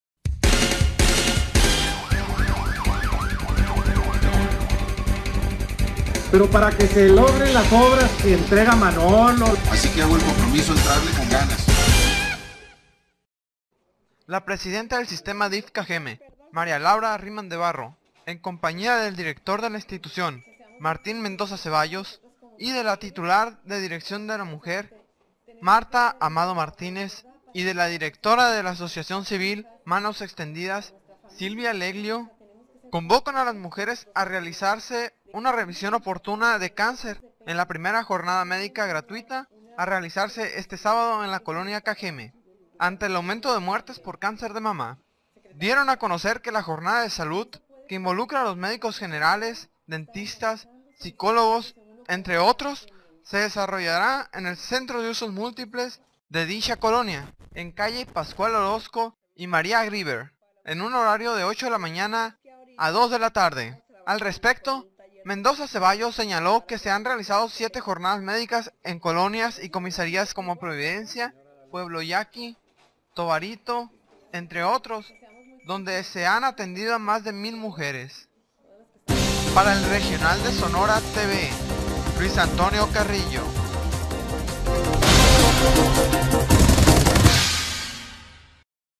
Iniciará la Primera Jornada Médica gratuita.
la Presidenta del Sistema DIF-Cajeme, María Laura Riemann de Barro; en compañía del Director de la Institución, Martín Mendoza Cevallos;  de la titular de la Dirección de la  Mujer, Martha Amado Martínez, y la Directora de la Asociación Civil, Manos Extendidas, Silvia Legliu, convocan a las mujeres a realizarse una revision oportuna de cáncer, en la Primera Jornada Médica gratuita a realizarse este sábado en la colonia Cajeme. Ante el aumento de muertes por cáncer de mama,
Dio a conocer que la jornada de salud que involucra a médicos generales, dentistas, psicólogos  y entre otras actividades, se desarrollará en el Centro de Usos Múltiples de dicha colonia en calle  Pascual Orozco y María Griver, en un horario de las 08:00 a las 14:00 hrs.
Al respecto, Mendoza Cevallos señaló que se han realizado siete Jornadas Médicas en colonias y comisarías como Providencia, Pueblo Yaqui, Tobarito y entre otros, donde se han atendido a más de mil mujeres.